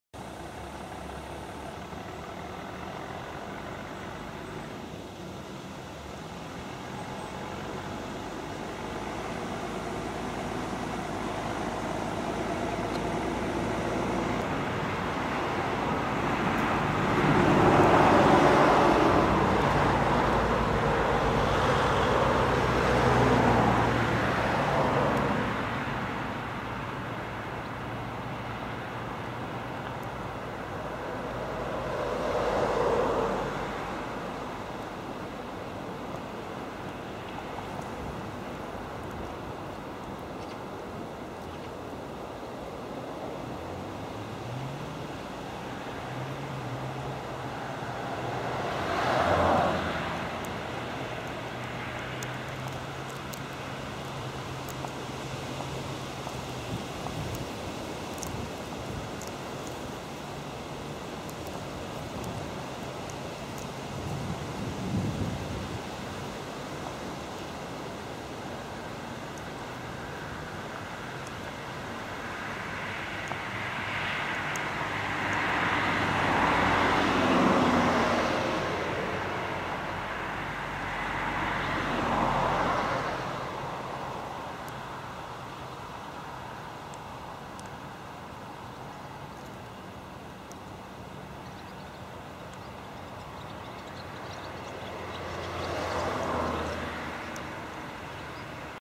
Zwei Störche in Hamburg-Harburg
Sie suchen Nahrung auf einer frischgemähten Wiese.

Am 8.8.12 in Hamburg - Harburg, Großmoordamm kurz vor Obi.